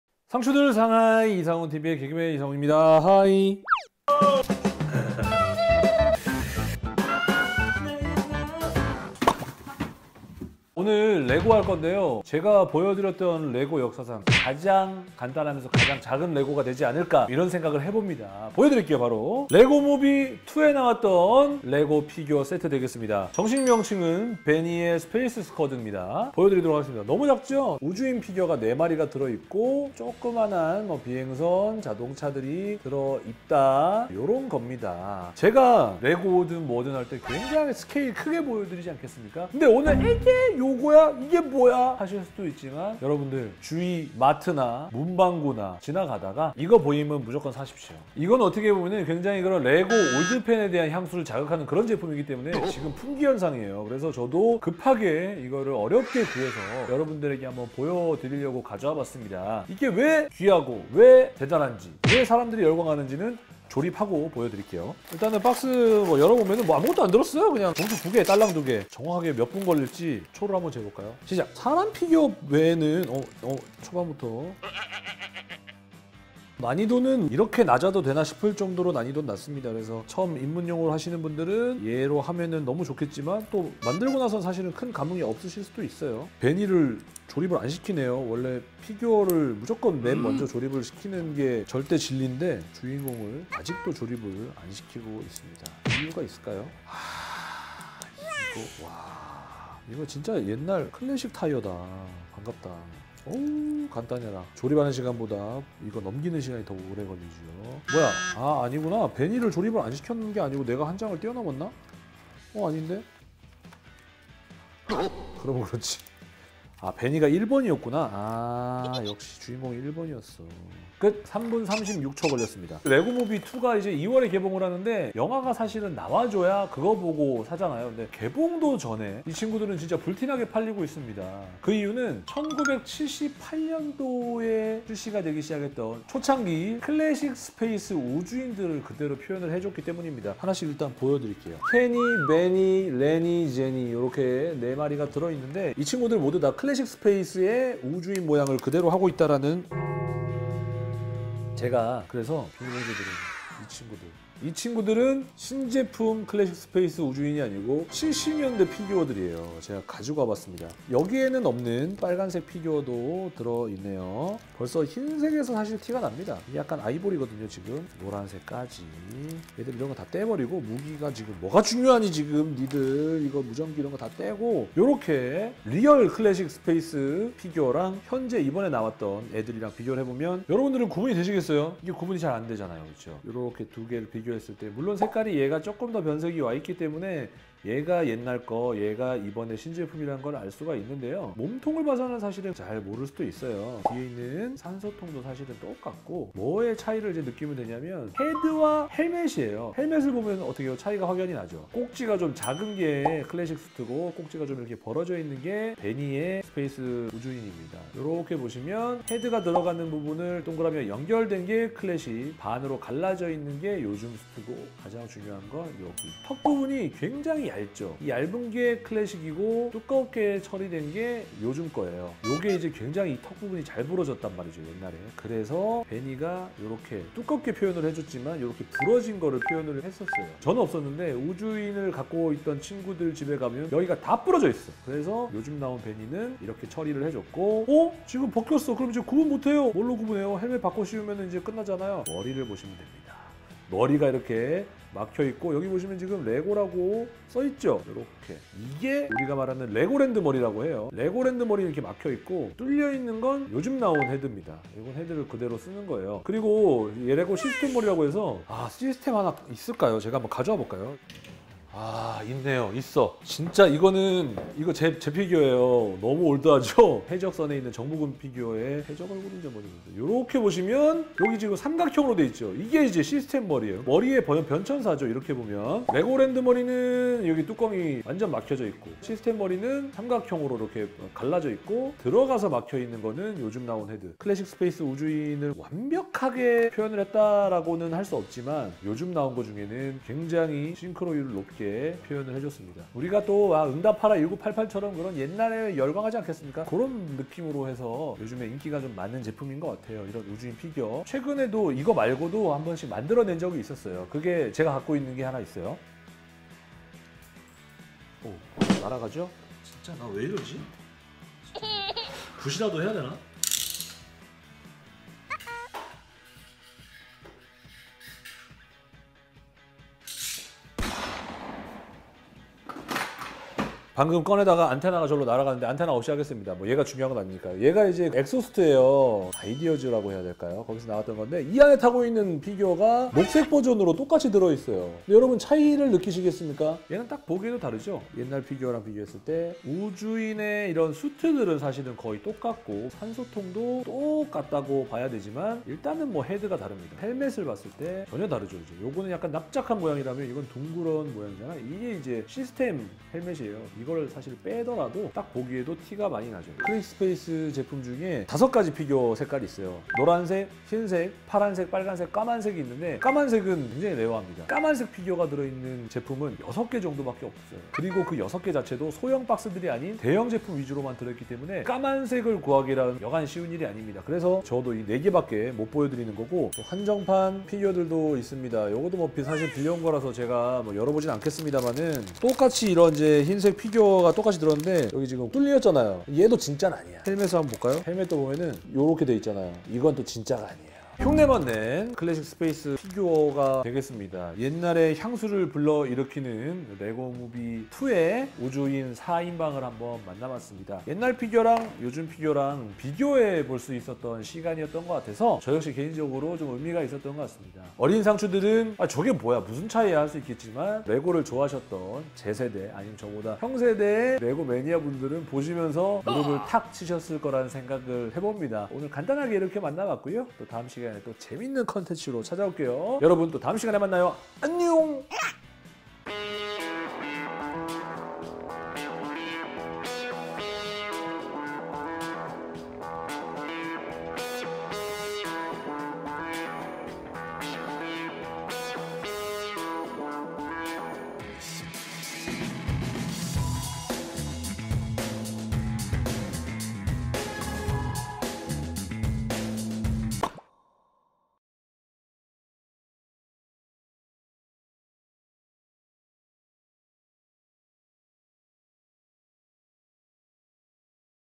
[LEGO] 올드 희귀 피규어! VS 품절대란 피규어!
올드레고 팬들에게 뜨거운 반응을 얻고 있는 70841~!
(Benny's space squad ) 그래서! 실제로 1978년도에 출시된
클레식 스페이스 피규어와 함께 비교 분석(?) 해보았습니다.